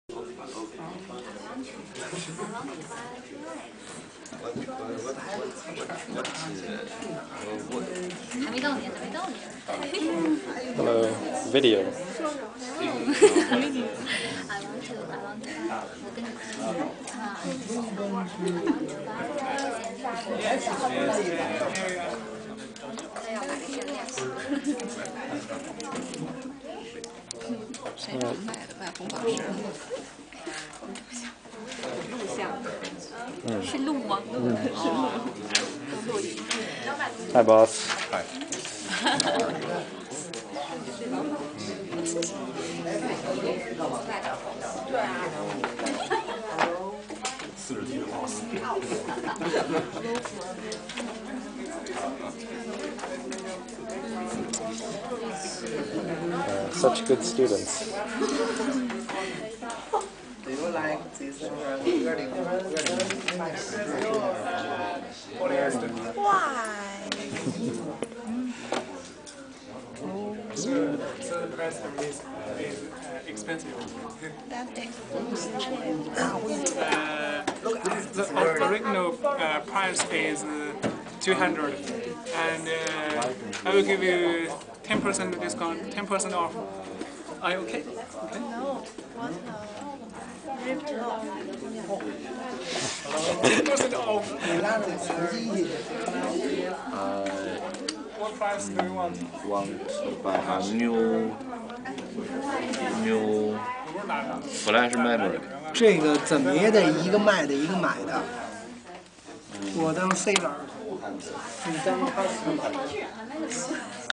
Students in action
One of my Chinese classes. These dudes work at a shopping center called Modern Plaza in Zhongguancun. I like em a bunch.